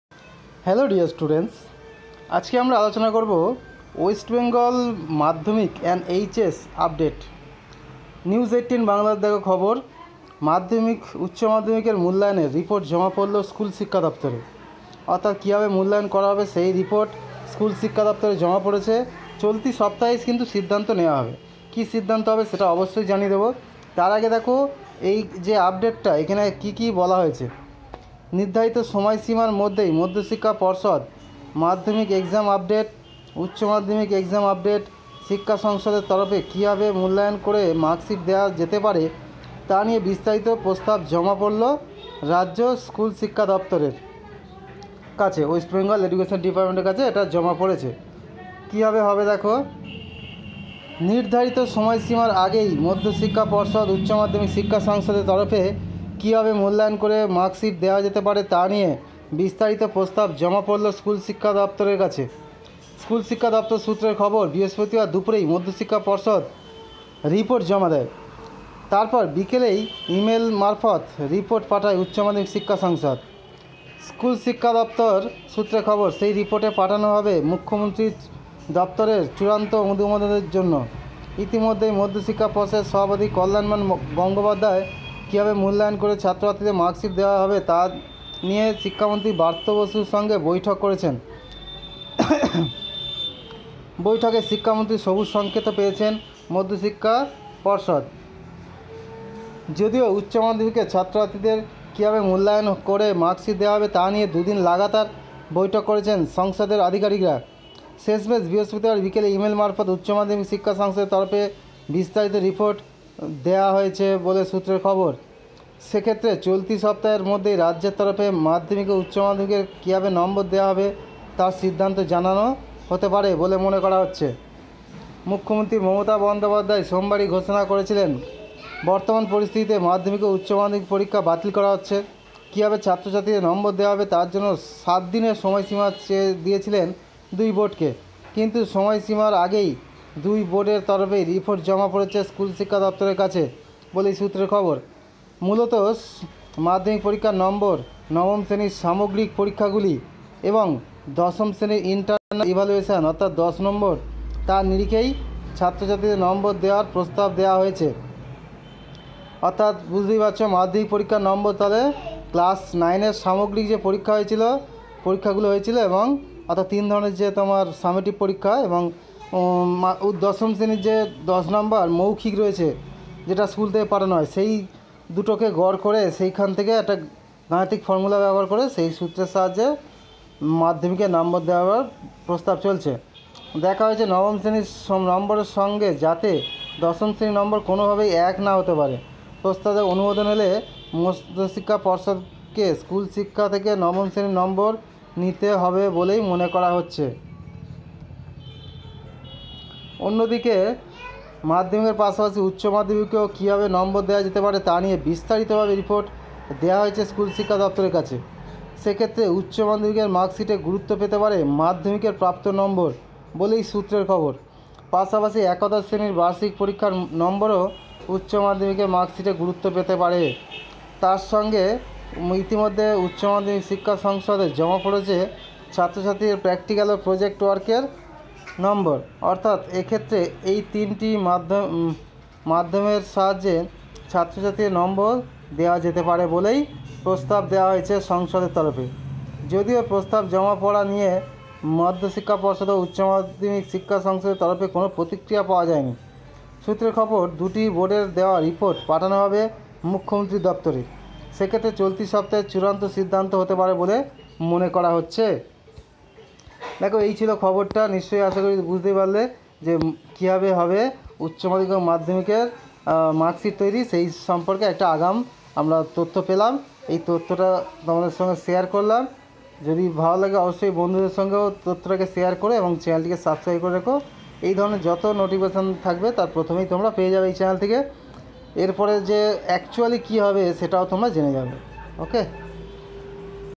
West Bengal Madhyamik and HS Update: মাধ্যমিক-উচ্চ মাধ্যমিকের মূল্যায়নের রিপোর্ট জমা পড়ল শিক্ষা দফ
West Bengal Madhyamik and HS Update: মাধ্যমিক-উচ্চ মাধ্যমিকের মূল্যায়নের রিপোর্ট জমা পড়ল স্কুল শিক্ষা দফতরে, চলতি সপ্তাহেই সিদ্ধান্ত?

নির্ধারিত সময়সীমার আগেই মধ্যশিক্ষা পর্ষদ (Madhyamik Exam Update), উচ্চমাধ্যমিক (HS Exam Update) শিক্ষা সংসদের তরফে কী ভাবে মূল্যায়ন করে মার্কশিট দেওয়া যেতে পারে তা নিয়ে বিস্তারিত প্রস্তাব জমা পড়ল রাজ্য স্কুল শিক্ষা দফতরের (West Bengal Education Department) কাছে।